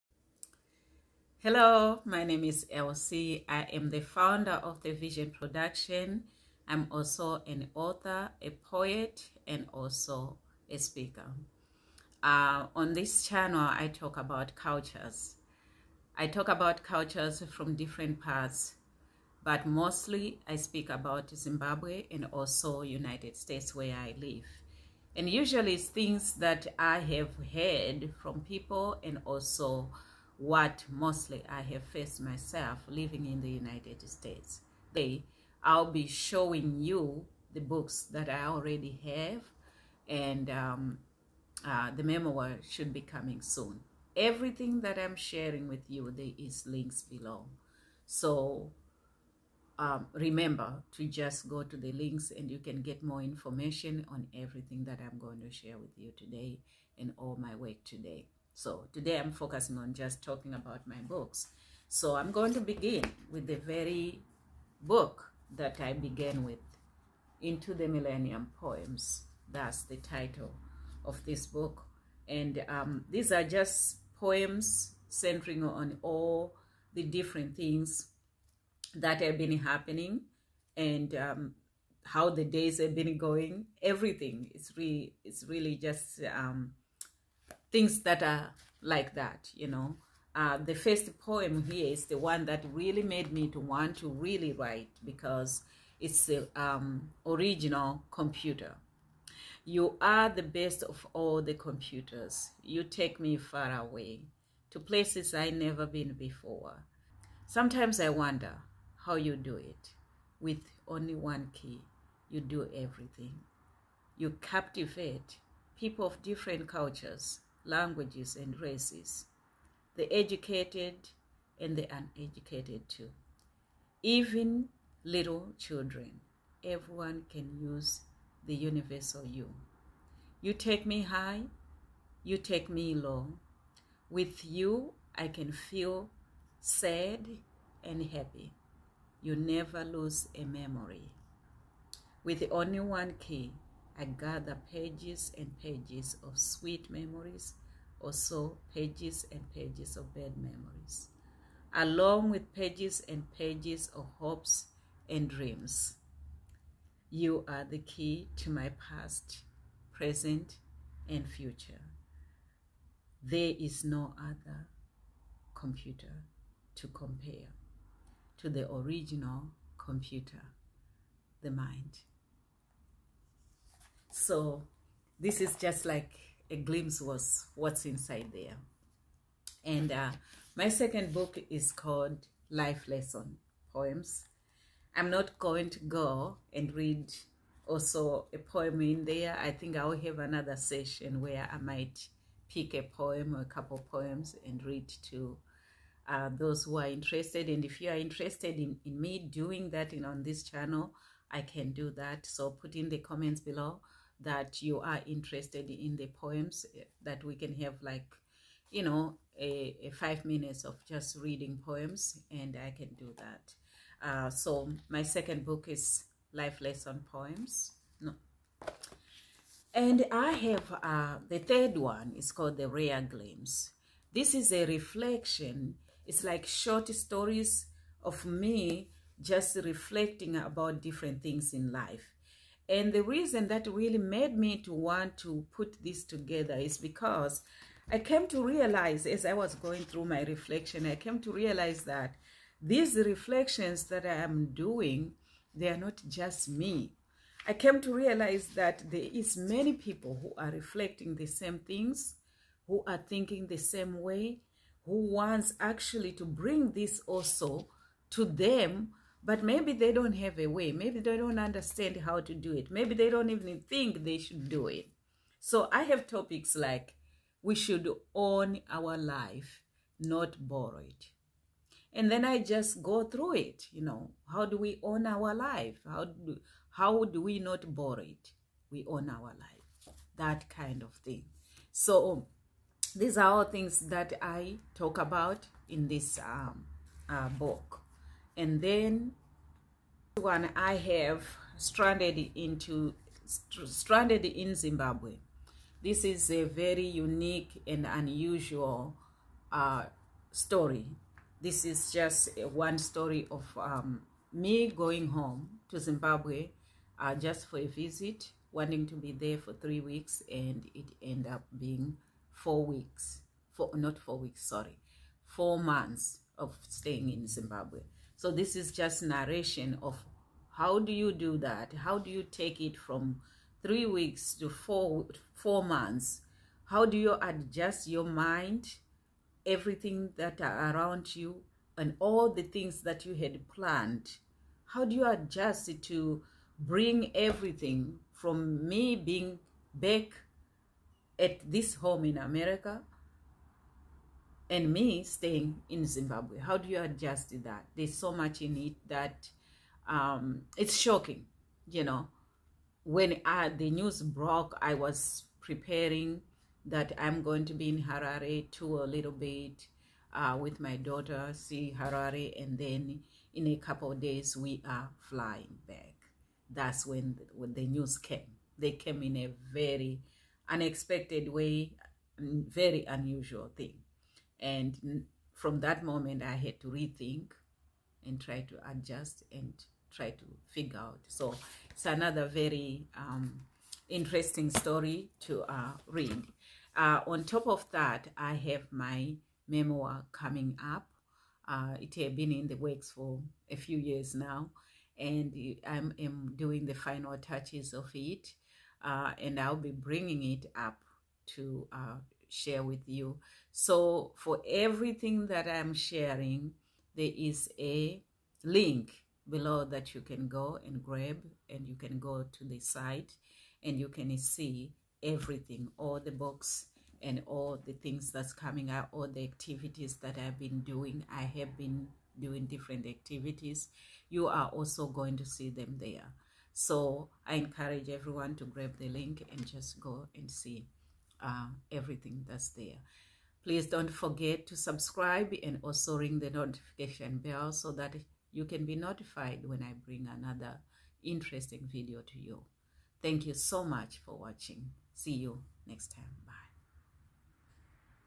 Am I an Author? Introducing my books
Am I an Author? I n this video I'm show casing my books, the work that I have been doing as a side hustle for over twenty years. I love to write and share different experiences in life. Life as a whole, Culture, traditions, personal stories and other stories including poetry